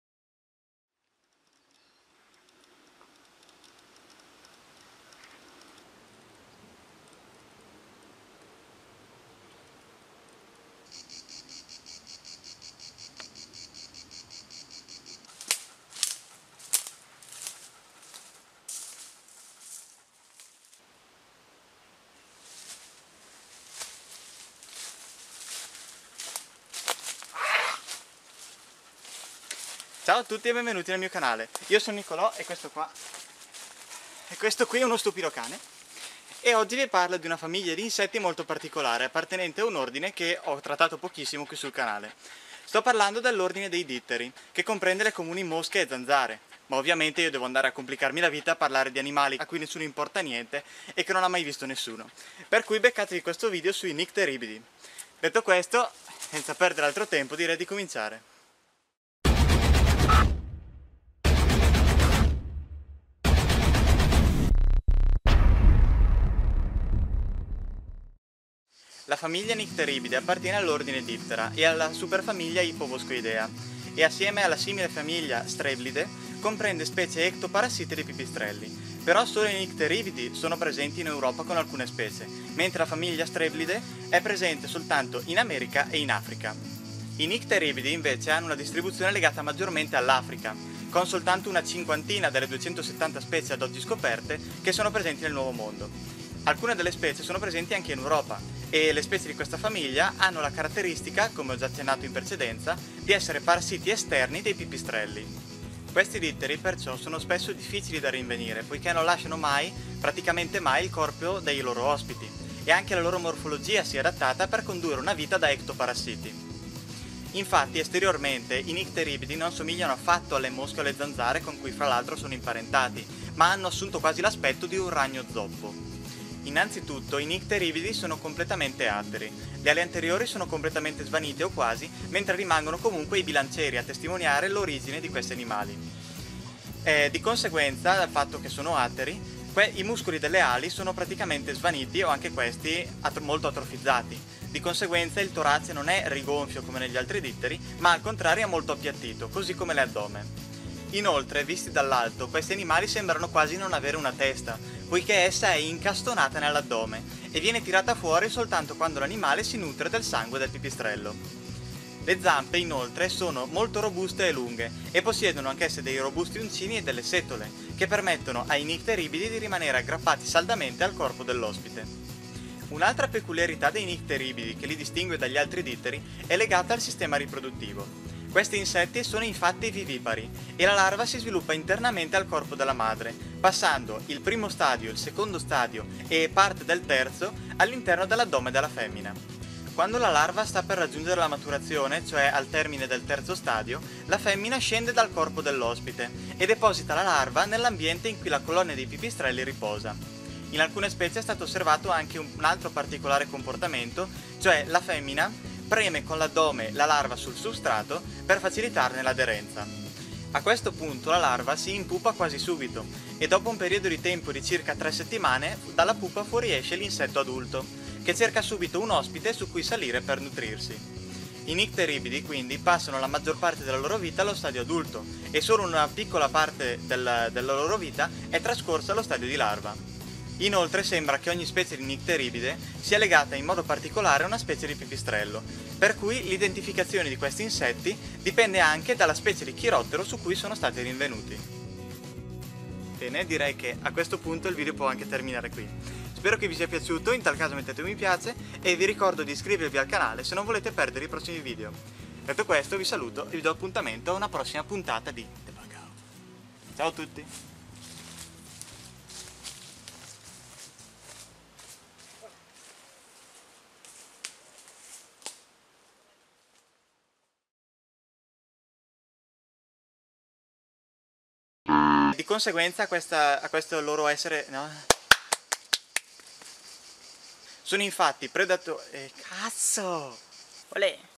(ITA | SUB ENG) Le mosche dei pipistrelli: Nycteribiidae - bat flies
Ciao a tutti! In questo video vi parlerò di un particolare famiglia di ditteri (anche se  difficilmente qualcuno a prima vista potrebbe accomunarli a una comune mosca o zanzara!), i nycteribiidi, chiamati anche mosche dei pipistrelli. Buona visione!